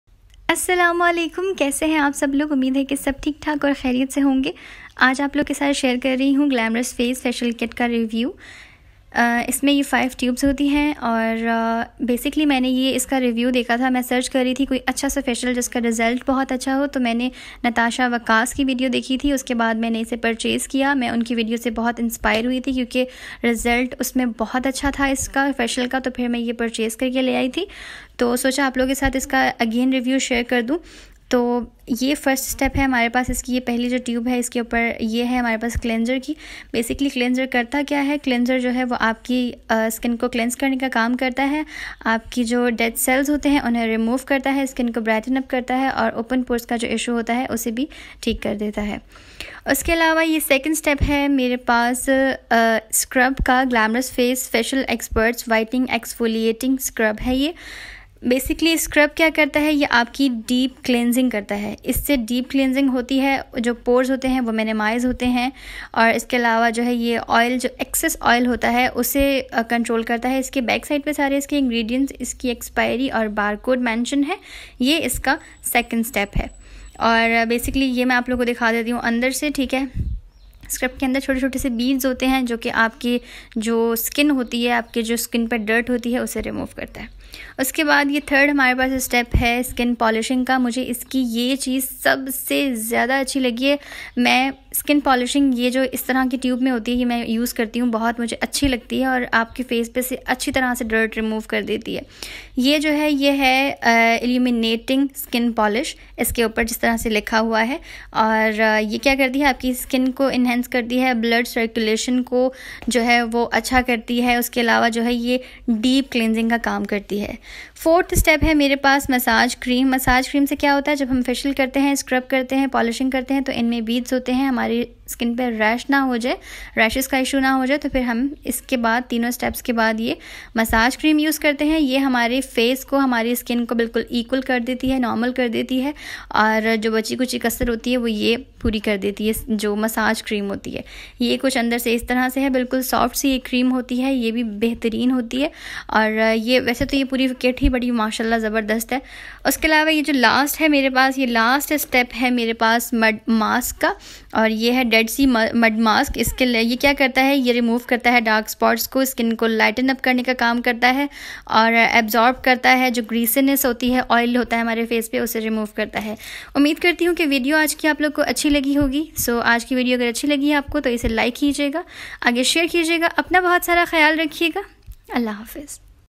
Glamorous face facial kit|| best whitening kit| inspired by natasha waqas review
Assalam o alaikum mera naam ujala hai aj apke liye best whitening and glowing facial kit ka review le kar aai hon jo k glamorous face ka hai main natasha waqas ki vdeo dekhnay k bd ye purchase kar k laai hon best facial hai reults bht zbrdst hen.